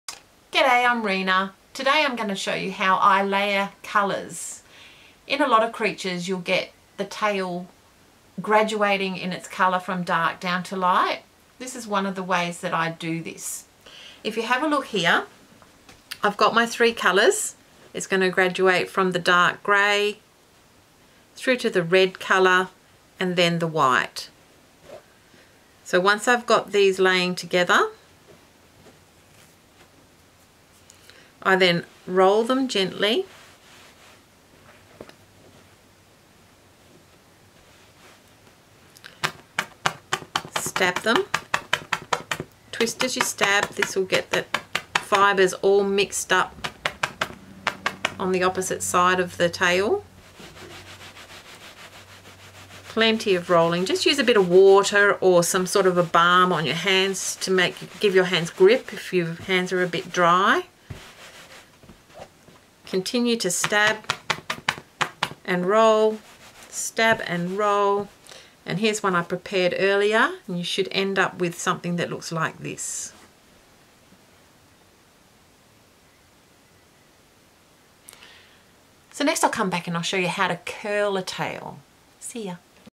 Needle felting layered colours
In this needle felting tutorial you will learn how to layer colours.